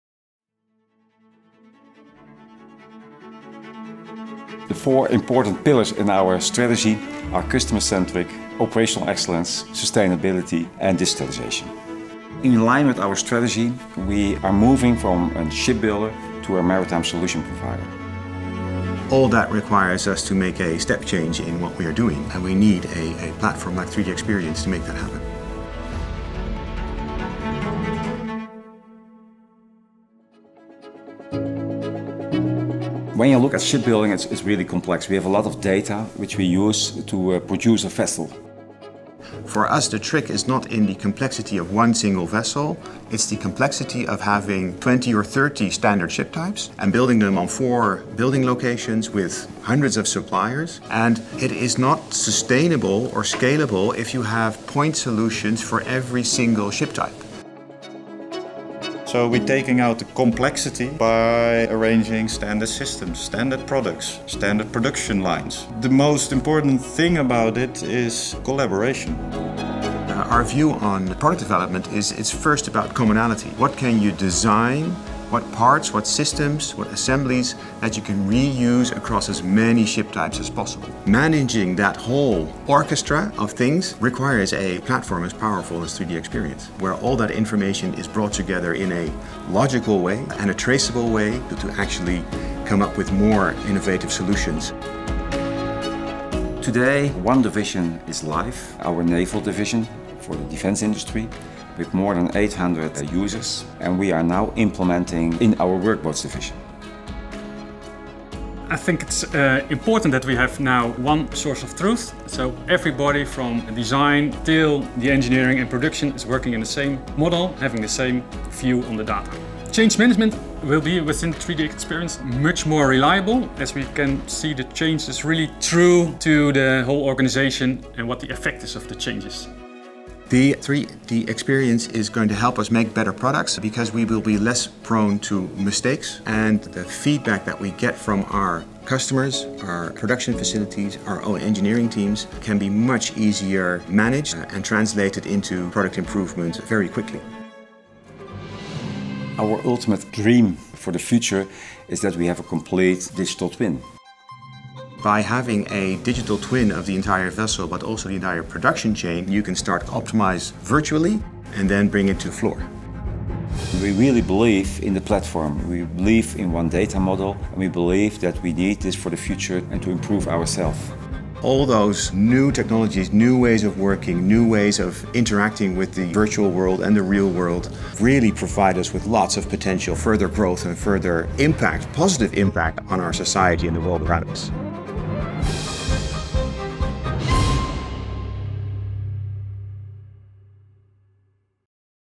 Damen Shipyards - Design, build and service anywhere ǀ Dassault Systèmes
Through a strong partnership with Dassault Systèmes, Damen Shipyards is implementing the 3DEXPERIENCE platform as the backbone for its vessels' lifecycle management, integrating design, engineering, and manufacturing engineering within a single, connected digital environment.

00:00 Becoming a maritime solution provider
00:31 Standardization to tackle complexity
01:21 Managing shipbuilding processes on the 3DEXPERIENCE platform
02:28 A virtual twin for future success

The four important pillars in our strategy are customer centric, operational excellence, sustainability and digitalization. In line with our strategy, we are moving from a shipbuilder to a maritime solution provider. All that requires us to make a step change in what we are doing, and we need a platform like 𝟯𝗗EXPERIENCE to make that happen. When you look at shipbuilding, it's really complex. We have a lot of data which we use to produce a vessel. For us, the trick is not in the complexity of one single vessel. It's the complexity of having 20 or 30 standard ship types, and building them on four building locations with hundreds of suppliers. And it is not sustainable or scalable if you have point solutions for every single ship type. So we are taking out the complexity by arranging standard systems, standard products, standard production lines. The most important thing about it is collaboration. Our view on product development it’s first about commonality – what can you design, what parts, what systems, what assemblies that you can reuse across as many ship types as possible. Managing that whole orchestra of things requires a platform as powerful as 𝟯𝗗EXPERIENCE where all that information is brought together in a logical way and a traceable way, to actually come up with more innovative solutions. Today, one Division is live, our Naval division for the Defense industry with more than 800 users. And we are now implementing in our Workboats division. I think it's important that we have now one source of truth. So everybody from design till the engineering and production is working in the same model, having the same view on the data. Change management will be within 𝟯𝗗EXPERIENCE much more reliable as we can see the changes really through to the whole organization and what the effects of the change is. The 𝟯𝗗EXPERIENCE is going to help us make better products because we will be less prone to mistakes. And feedback that we get from our customers, our production facilities, our own engineering teams can be much easier managed and translated into product improvement very quickly. Our ultimate dream for the future is that we have a complete digital twin. By having a digital twin of the entire vessel but also the entire production chain, you can start first optimize virtually and then bring it to the floor. We really believe in the platform. We believe in one data model, and we believe that we need this for the future and to improve ourselves. All those new technologies, new ways of working, new ways of interacting with the virtual world and the real world really provide us with lots of potential for further growth and further impact, positive impact on our society and the world around us.